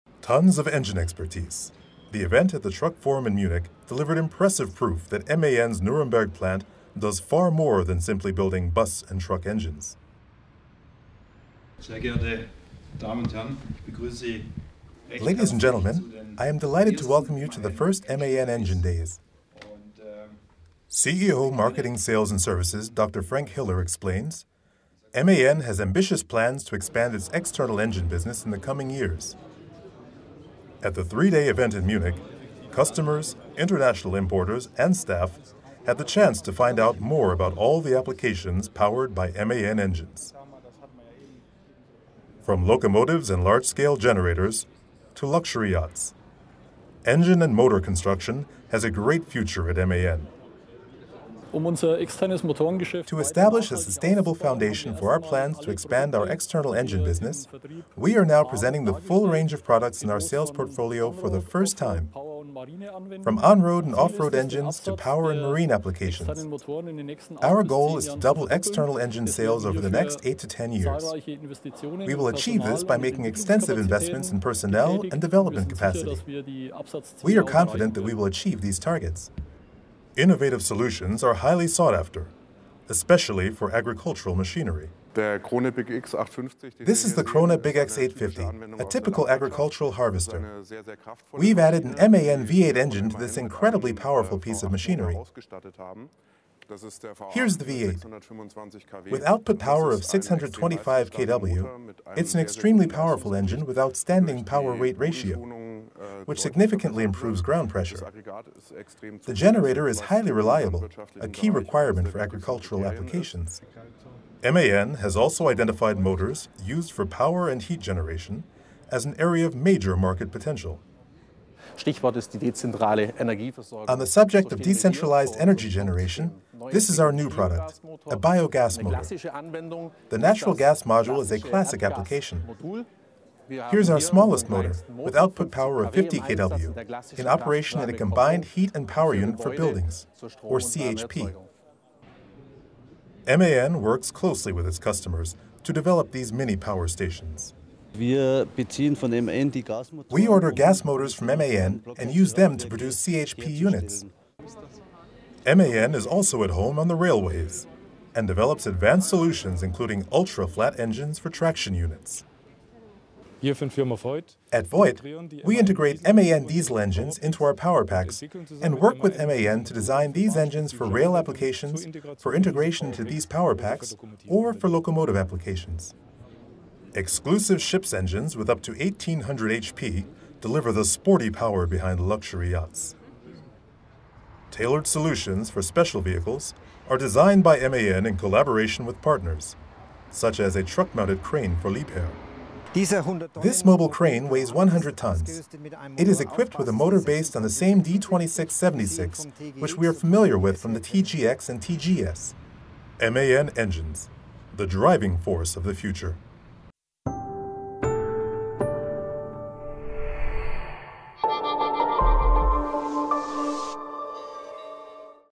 Engine Days - MAN powers up its engine operations
The full range of MAN engine and motor products could be admired at the Engine Days, held at the Truck Forum in Munich on May 11th.